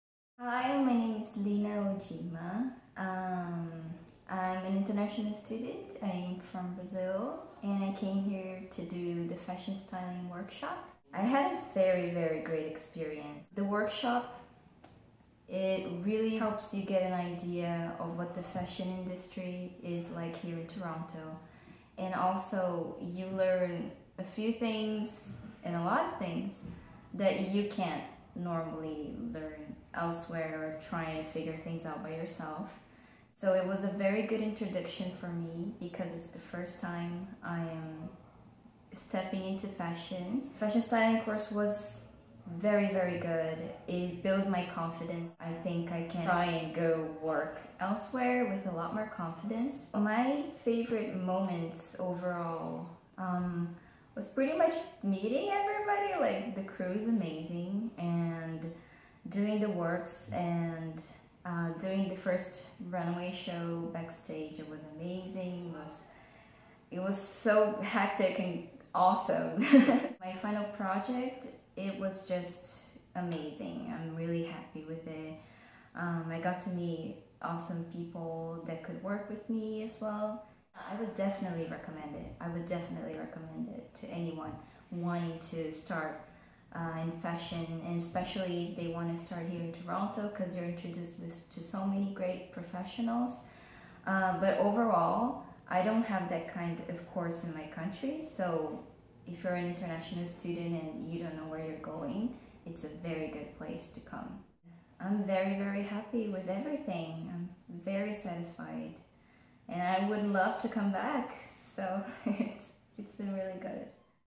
Lina O., Fashion Styling Graduate Testimonial
Lina Ogima, Fashion Styling Graduate

Portfolio Work:

1. Flora Femina High-Fashion Editorial 2016
2. Seasonz Toronto SS17 Look-book Shoot
3. Precious by Maira - Jewelry Editorial 2016
4. Asst. Stylist, Chic Editorial for Claudia E. 2016
5. Diwali Festival Gala Fashion Show 2016
6. Select Fashion Night Fashion Show 2016
7. Richmond Hill Tastes of the Hill Fashion Show 2016
8. Styled Models for Commercial & Editorial Photo-shoots